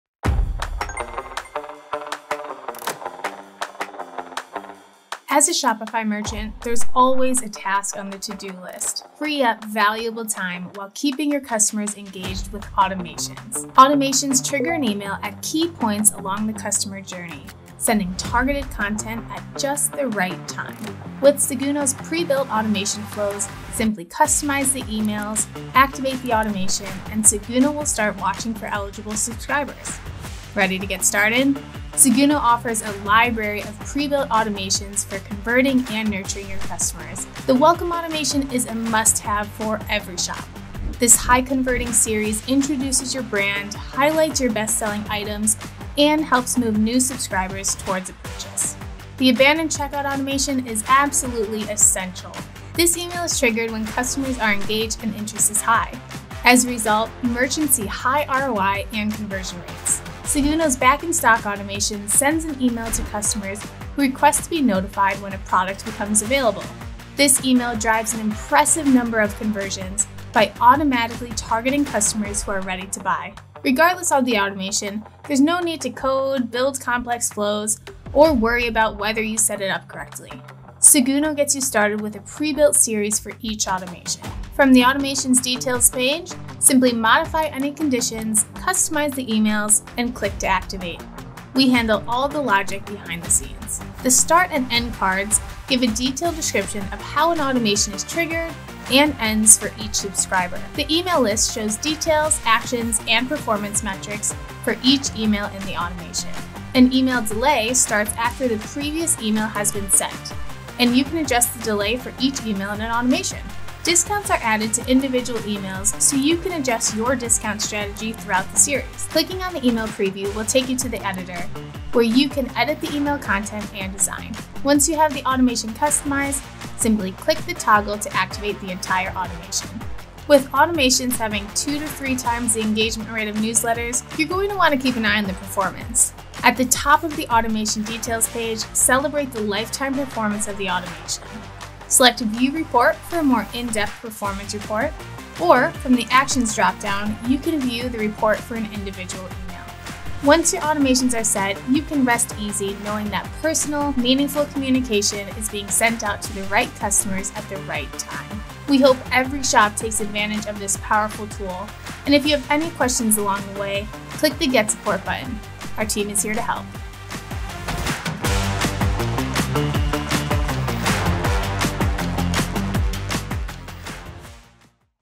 Getting Started with Automations
The video introduces the concept of automations for customer engagement, focusing on email triggers at key points in the customer journey. It highlights the ease of customization and activation of pre-built automation flows, such as welcome and abandoned checkout automations. The text emphasizes the absence of coding and complex setup, allowing for customization of conditions, emails, and activation. It also mentions the performance metrics, adjustable email delay, and the option to add discounts to individual emails. The video encourages users to take advantage of the powerful tool and seek support if needed.

00:00 Open
00:13 What is an automation
00:20 Seguno pre-built automation flow
00:32 Seguno Automations
00:37 Welcome Automation
00:50 Abandoned Checkout Automation
01:00 Back-in-Stock Automation
01:24 Modify Automations
01:34 Start and End Cards
01:40 Email List
01:46 Email Delay
02:16 Automation Performance Reports